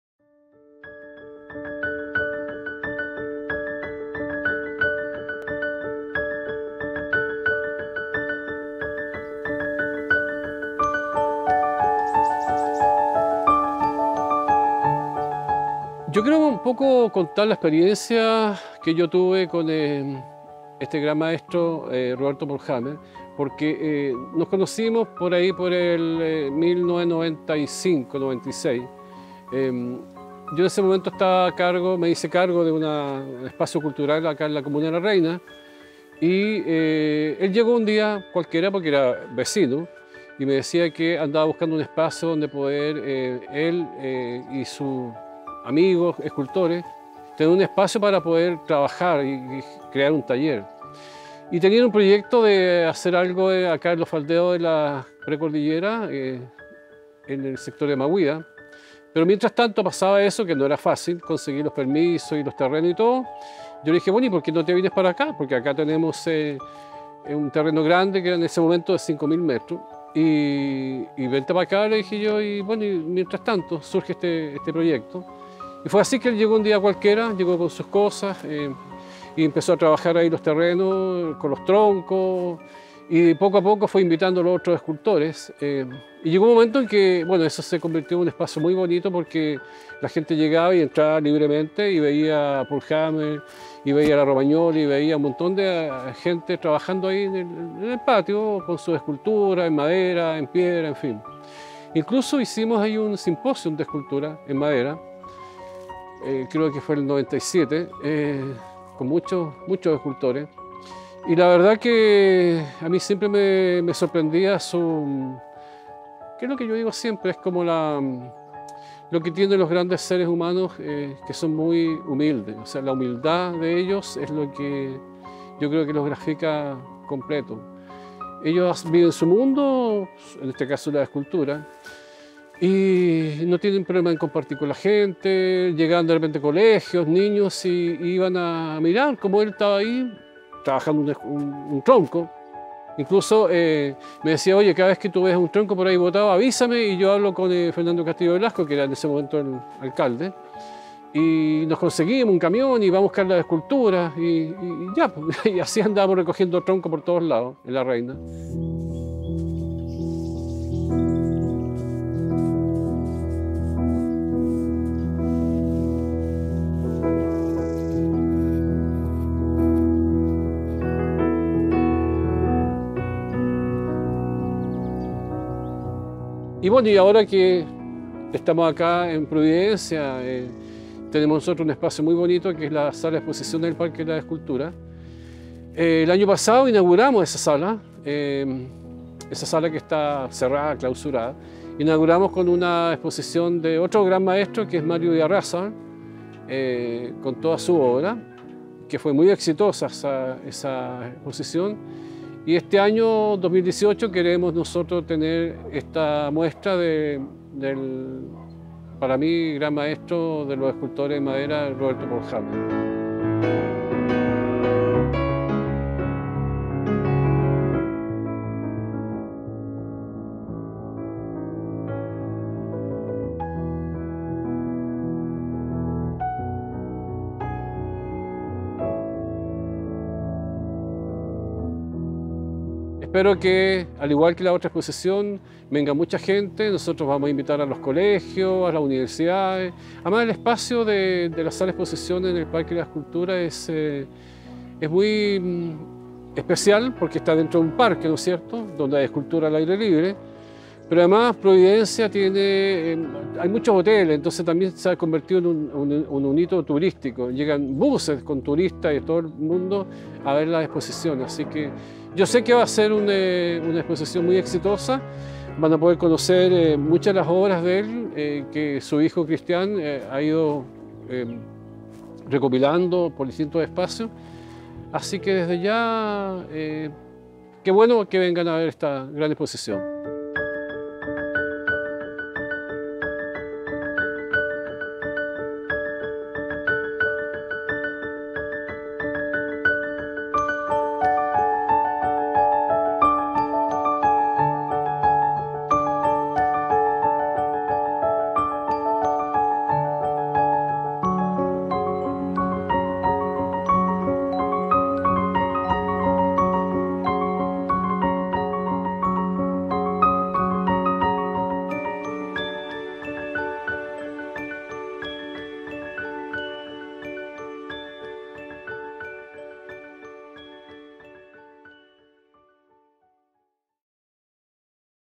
Roberto Pohlhammer "El Espíritu de la Madera" Micro Documental (Sobre Exposición Galeria
Micro documental sobre la vida y obra de Roberto Pohlhammer (Versión corta), que se expresa a través de la exposición de su obra en la Galería del Parque de las Esculturas en la Comuna de Providencia.  Esta exposición sucede como hilo conductor de este micro documental.

Manuel Ahumada, director ejecutivo de la Fundación Providencia habla sobre su experiencia con Roberto Pohlhammer. Los inicios del taller de Escultores del parque Mahuida, en la Comuna de La Reina, entre otras anécdotas.

Un micro documental emocionante, que habla de la vocación del ser cuando encuentra su pasión. Habla sobre un hombre dedicado a la expresión a través de la madera, expresiones del espíritu, del dolor, de la mujer.

Agradecimientos a: La Fundación Providencia por su compromiso con la obra de Roberto Pohlhammer. Por su participación desde comienzo a final. Por la oportunidad de exhibir la obra de Roberto como se merece, en una galería impecable.

Director: Cristóbal Pohlhammer
Camara: Alonso Fernandez
Audio: Diego Rencoret
Montaje: Cristóbal Pohlhammer, David Cofre
Entrevistado: Manuel Ahumada